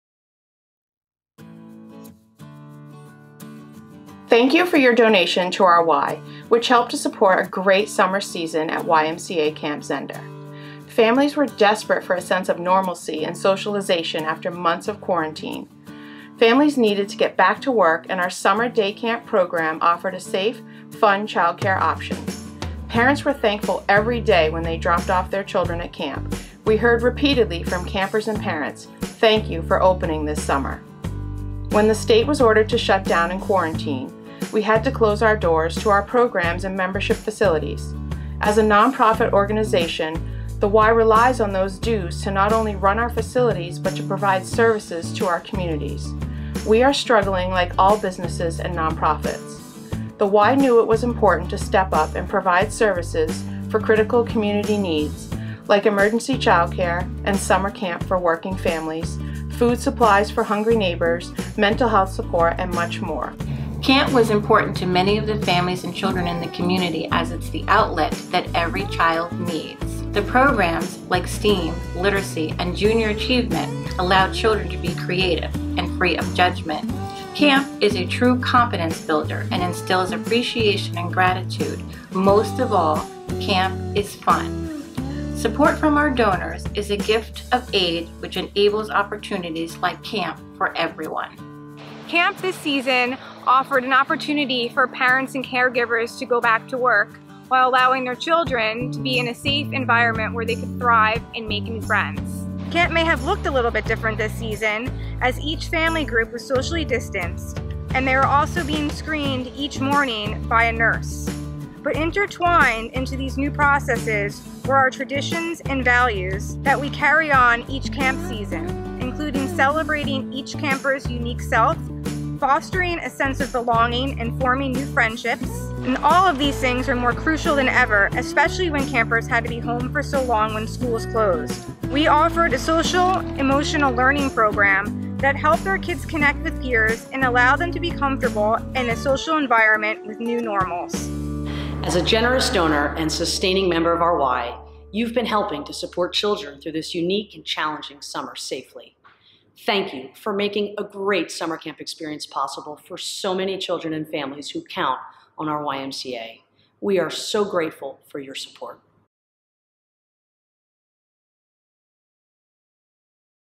Thanks for Making YMCA Summer Camp Possible
Thank you to our donors who helped make summer camp at the YMCA of Greater Monmouth County a possibility!